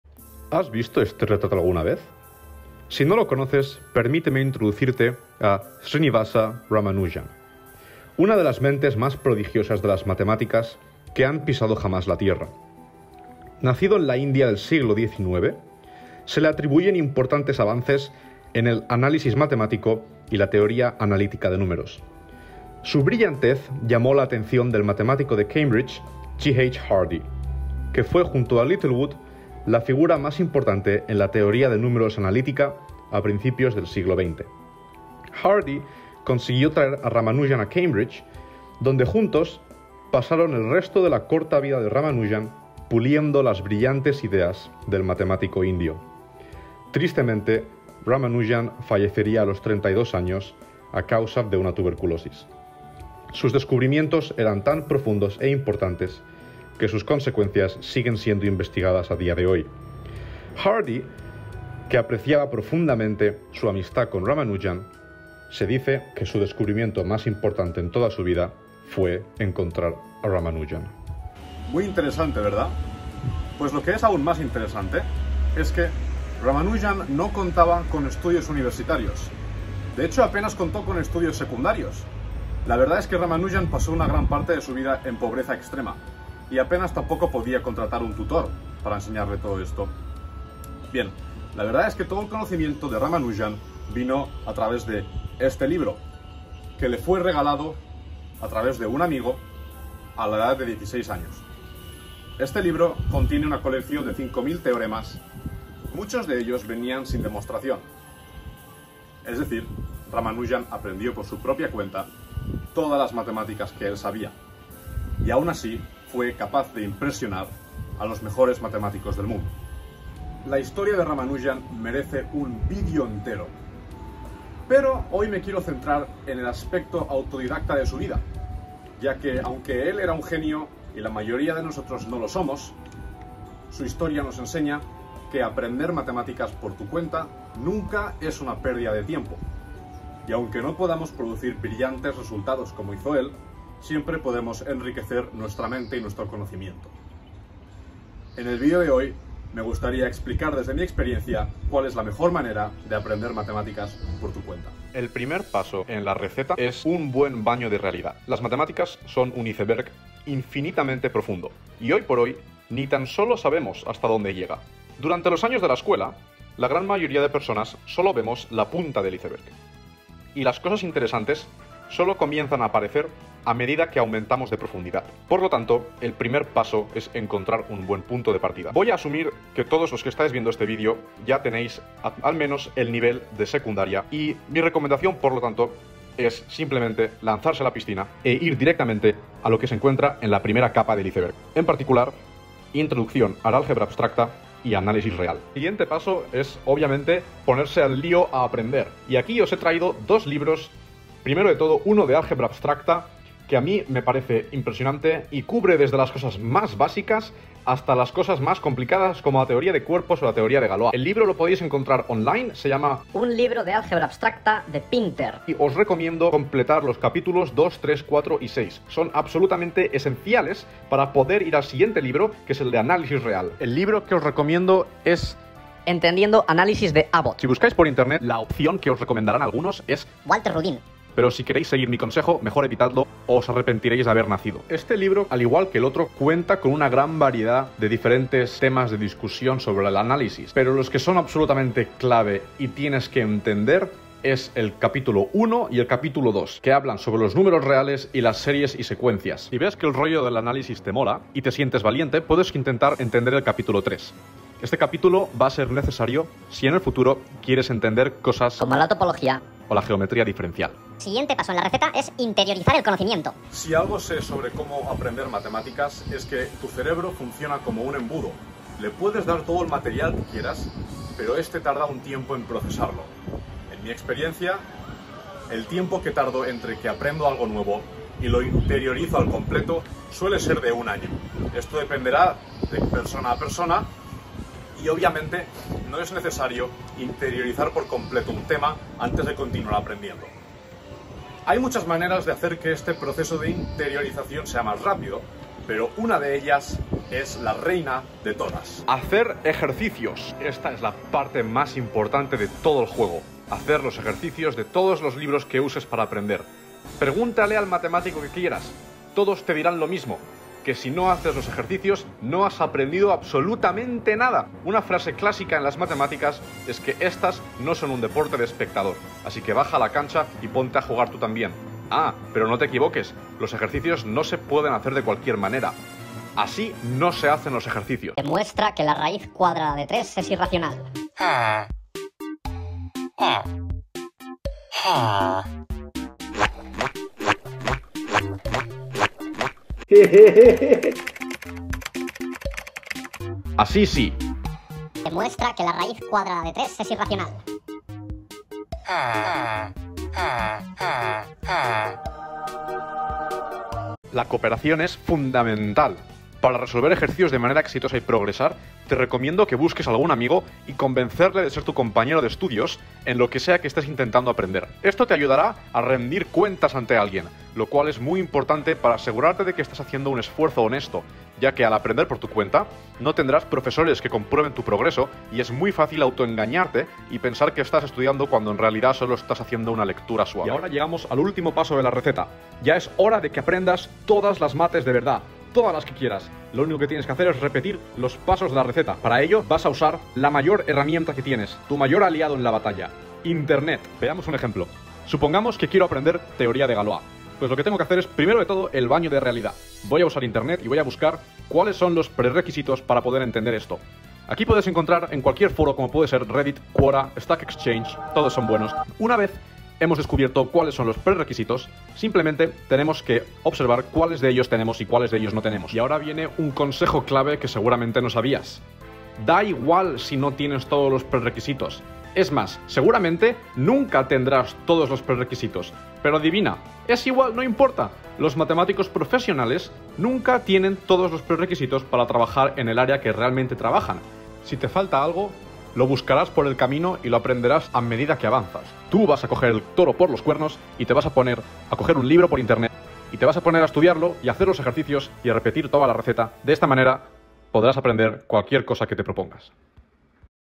Cómo aprender Matemáticas por tu cuenta y no morir en el intento
En este vídeo corto, hablo sobre mis consejos para aprender existosamente matemáticas por tu cuenta